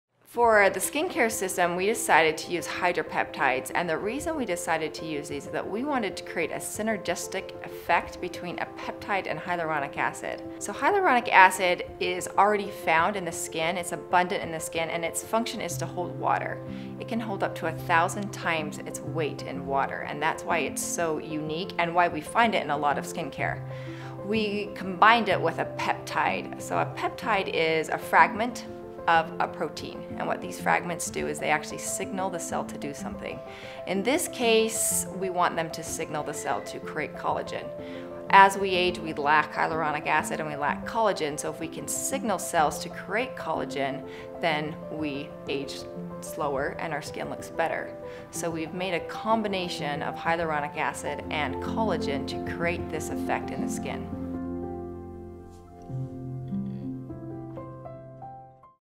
PMD Beauty | Hydrapeptides
Hydrapeptides are a synergistic combination of two important ingredients; hyaluronic acid and peptides. Hyaluronic Acid retains moisture in the skin, while peptides signal the cells in your skin to produce collagen. Hydrapeptides are included in each all of our skincare products to keep your skin hydrated and youthful.